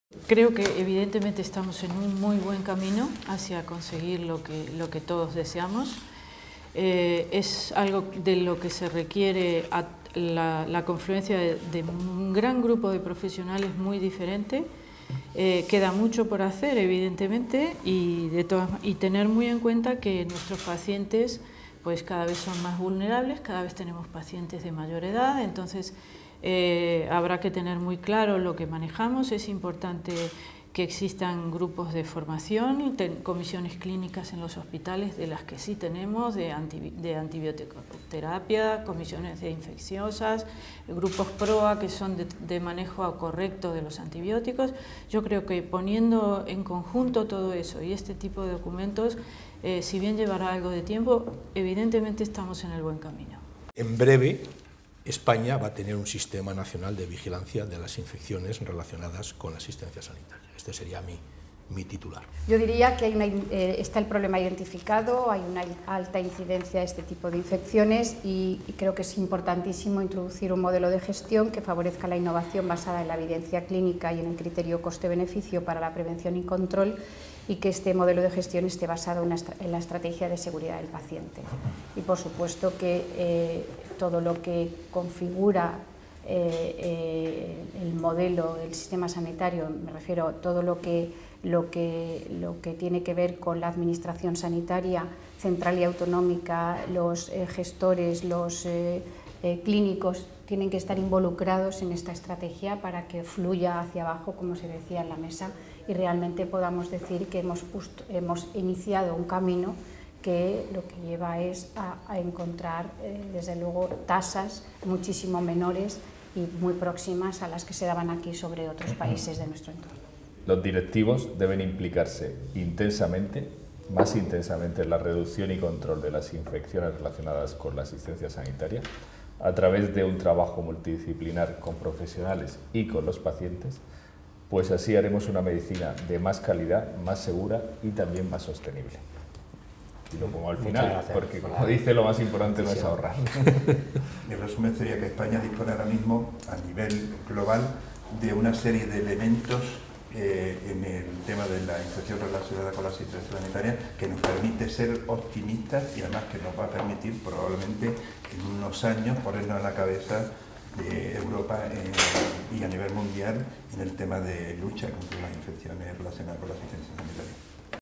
Cada ponente aporta un titular que sintetiza su posición sobre las IRAS
Las IRAS dispondrán de un observatorio en España en breve. Los ponentes coinciden en que, pese a los obstáculos que se esperan para su control, el problema se encuentra, como mínimo, muy bien delimitado.